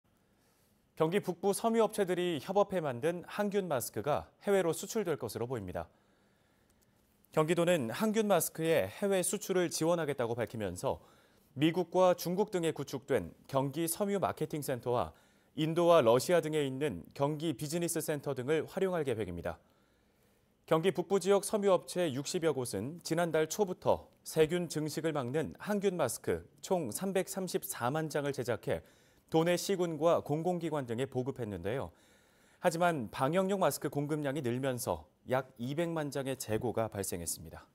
경기북부 항균 마스크, 해외 수출 준비
1. 타결 직전에 결렬···무급휴직 장기화?
2. 의정부 호원동 상가 화재···인명피해 없어
3. 코로나19 드라이브 스루 선별진료소 잠정 중단